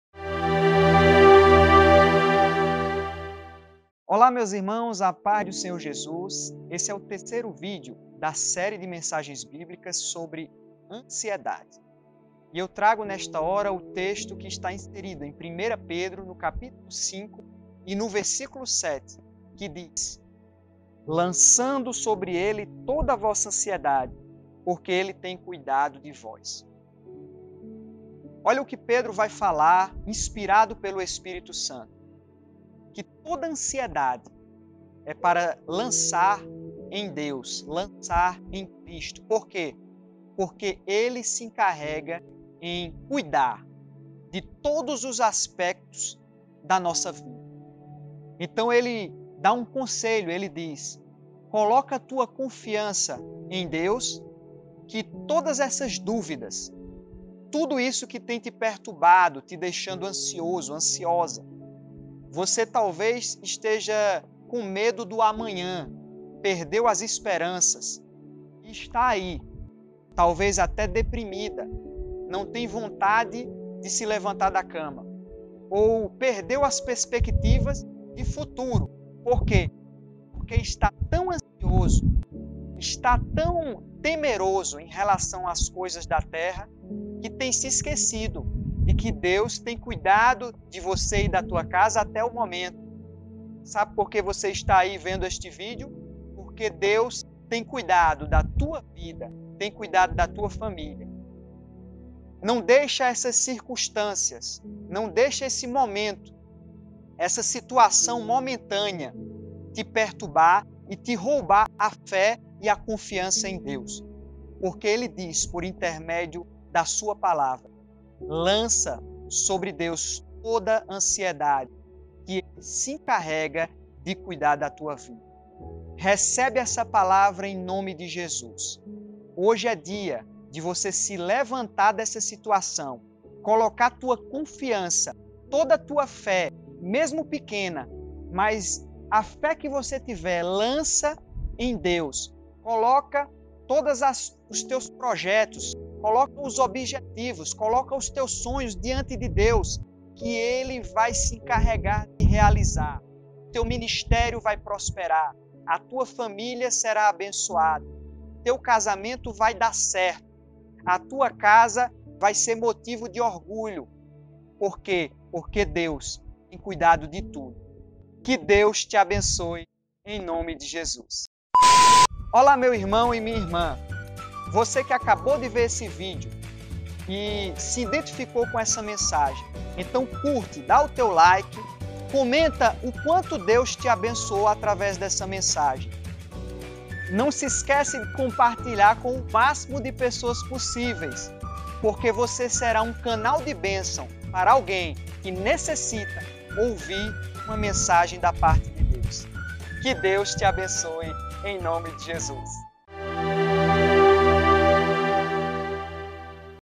HUGO GUIMARÃES | VÍDEO 3 (ANSIEDADE)
A palavra de Deus em 1 Pedro 5, 7 vai nos ordenar a lançar sobre Cristo a nossa ansiedade, ou seja, tudo aquilo que nos deixa ansioso, seja por preocupação nas coisas terrenas, ou por falta de esperança no amanhã, ou por falta de confiança em Deus

São aspectos na vida do ser humano que se não tivermos um entendimento espiritual, podem tirar a nossa paz e nos perturbar.

Portanto, essa mensagem nos conforta, pois vemos que Deus tem cuidado de nós em cada detalhe e se confiarmos nele, teremos paz.

Que Deus te abençoe em nome de Jesus!